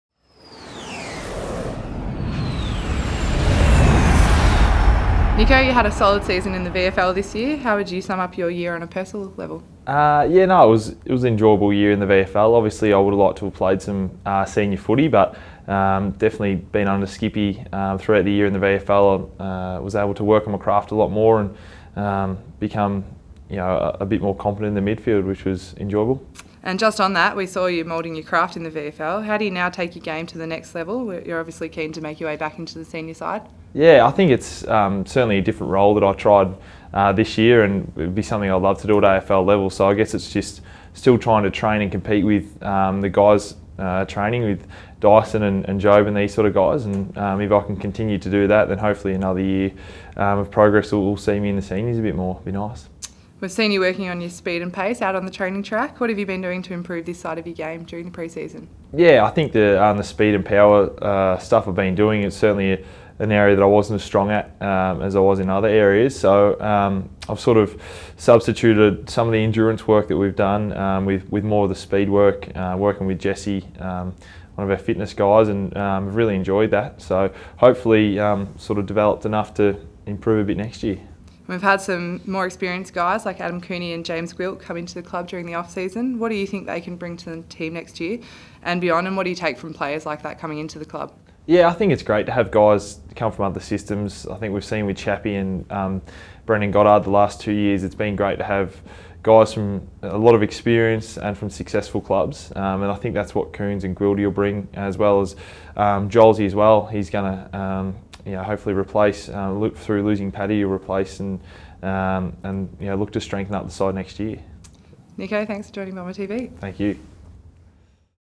BTV: Nick O'Brien - 28 December, 2014
Nick O'Brien reflects on his 2014 and looks forward to next year.